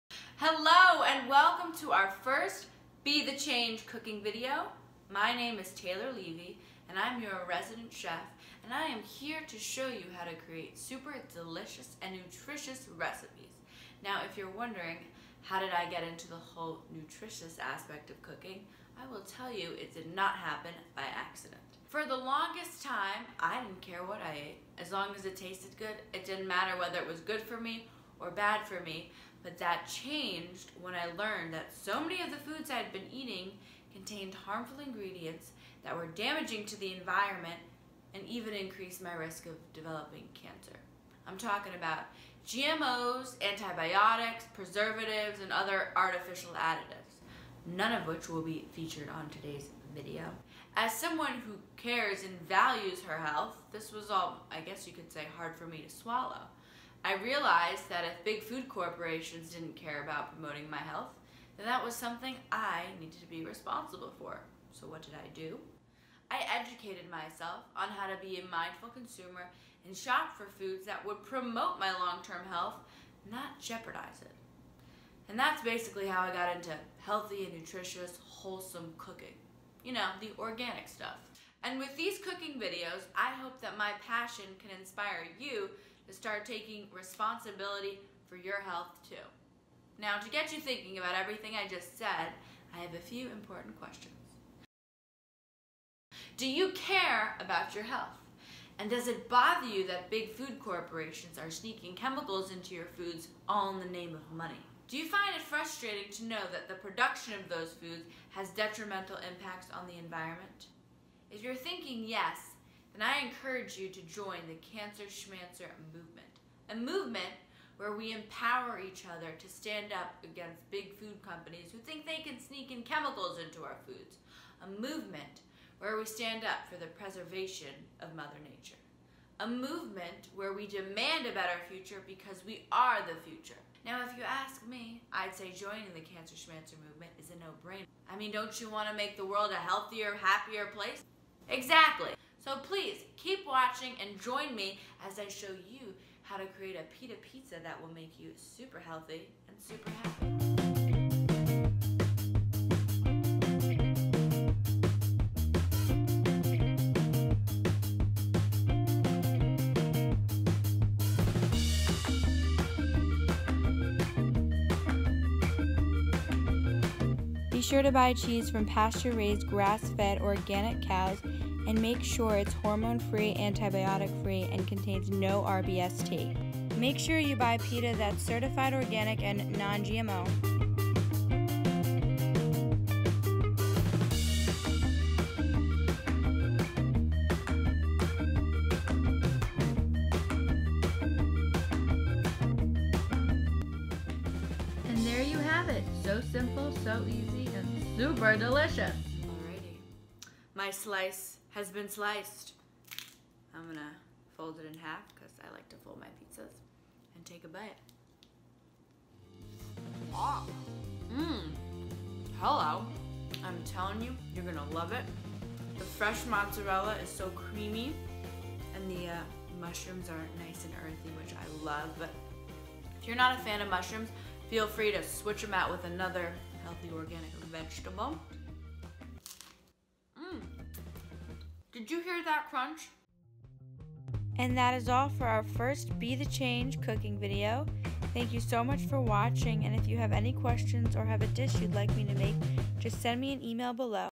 Healthy Pita Pizza Recipe from Taysteebites
Cancer Schmancer is thrilled to welcome chef extraordinaire Taylor Levy of Taysteebites to the WTF youth program team. Check out this delish, healthy, and easy-to-make recipe for pita pizza.